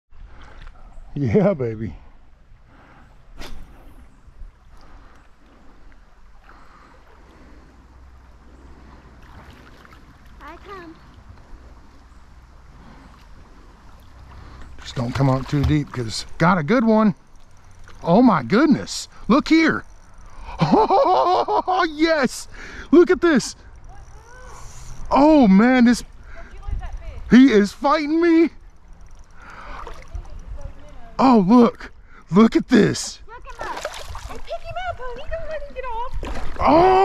This smallmouth is going to haunt me..........
we where wading and fishing on the 4th of July, I was catching little smallmouth and having a blast. I threw right in front of a large rock into a deep hole and hooked up with a monster (for a creek bass) as soon as I went to land it he spit the lure. this one hurts#fishing